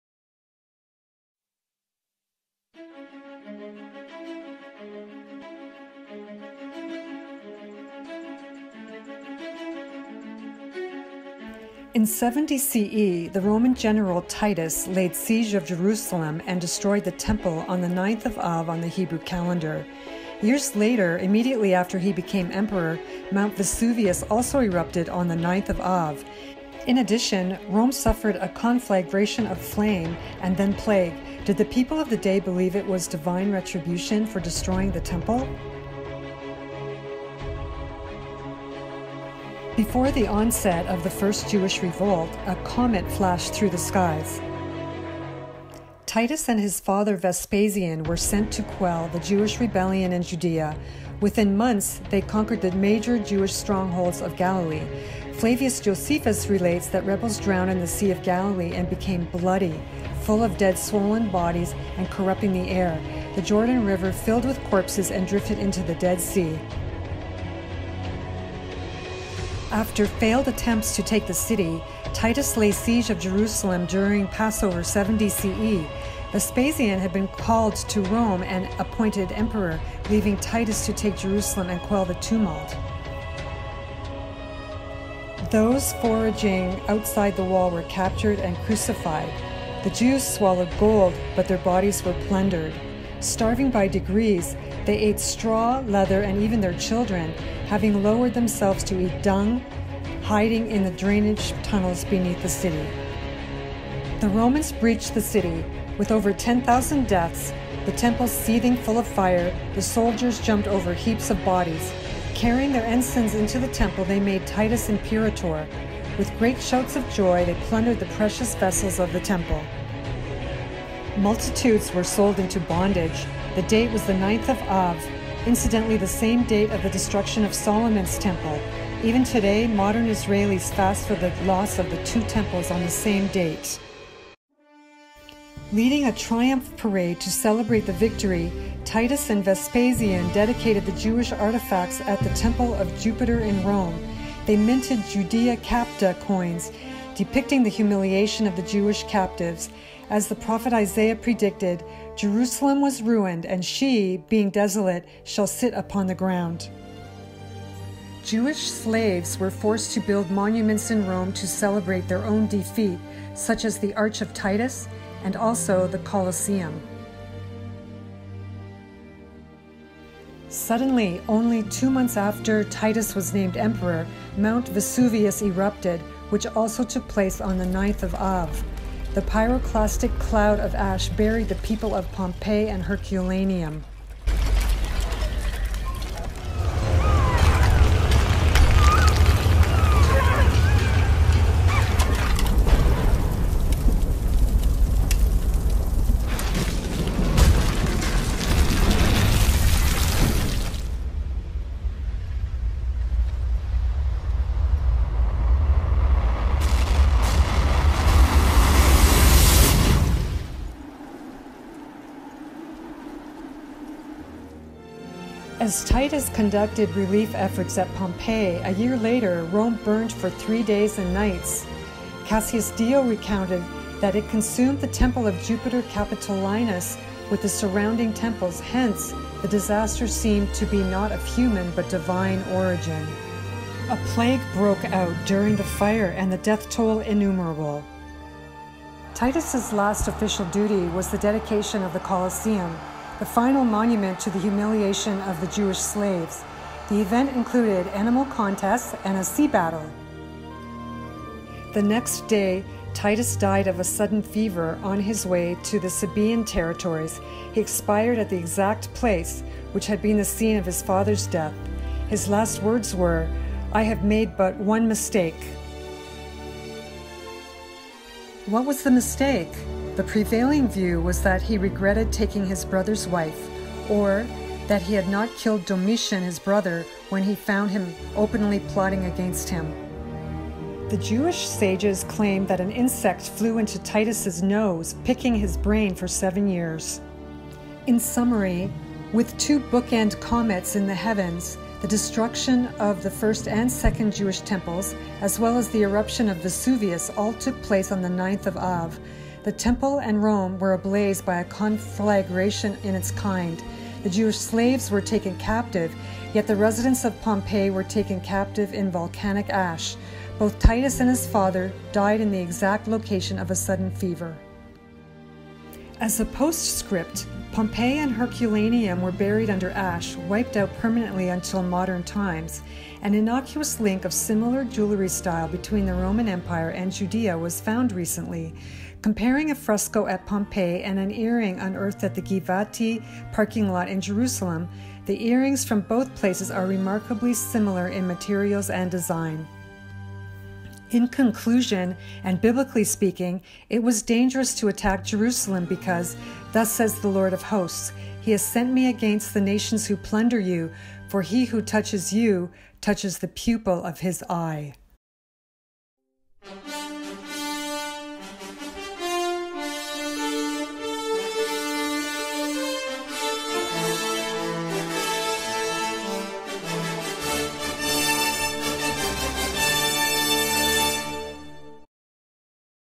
Pompeii: Divine Payback for Destroying Jewish Temple 70 CE?
Comets? Conflagration at Rome? Volcanic Eruption of Vesuvius? Vespasian and his son each die of sudden fever in separate events, but at the same place? Jewish captives build the Arch of Titus and the Coliseum, while the people of Pompeii were taken captive by a pyroclastic cloud. What did first sources say about the events, and was there a connection to the destruction of the Jewish Temple in Jerusalem in 70 CE? And what about Titus, who was responsible? What did the Rabbinic sages say about it? Events seem to defy an explanation.

Images in this production are often representational and not the actual location or the era:
i.e. The cottage where Vespasian and Titus died at Aquae Cutiliae.